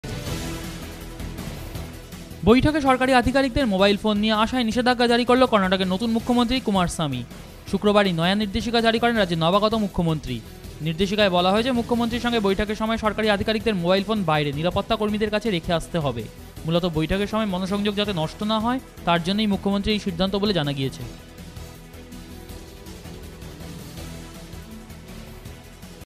বৈঠকে নিষিদ্ধ মোবাইল
বৈঠকে সরকারি আধিকারিকদের মোবাইল ফোন নিয়ে আসায় নিষেধাজ্ঞা জারি করল কর্ণাটকের নতুন মুখ্যমন্ত্রী কুমারস্বামী। শুক্রবার এই নয়া নির্দেশিকা জারি করেন রাজ্যের নবাগত মুখ্যমন্ত্রী। নির্দেশিকায় বলা হয়েেছ, মুখ্যমন্ত্রীর সঙ্গে বৈঠকের সময় সরকারি আধিকারিকদের মোবাইল ফোন বাইরে নিরাপত্তা কর্মীদের কাছে রেখে আসতে হবে। মূলত বৈঠকের সময় মনোসংযোগ যাতে নষ্ট না হয় তার জন্যই মুখ্যমন্ত্রীর এই সিদ্ধান্ত বলে জানা গিয়েছে।